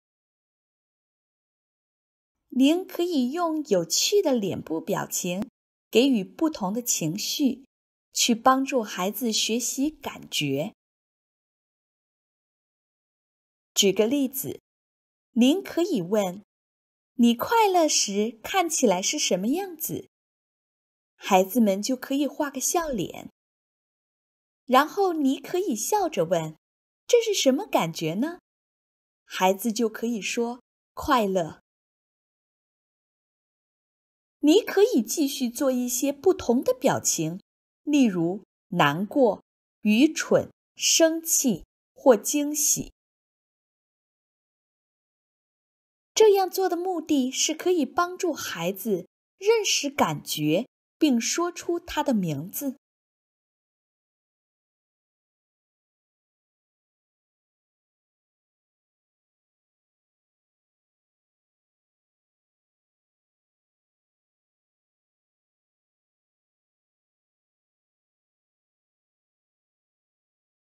感觉的名称 - Name the Feeling (Chinese)
你可以用有趣脸部表情给不同的情绪，去帮助孩子学习”感觉“。例如，快乐，难过，愚蠢，生气，和惊喜。

Chinese translation by Ming Liu, and Xueni Peng. Narration by Xueni Peng.

You can help a child learn about feelings with fun facial expressions for different emotions. For example, happy, sad, silly, mad, and surprised.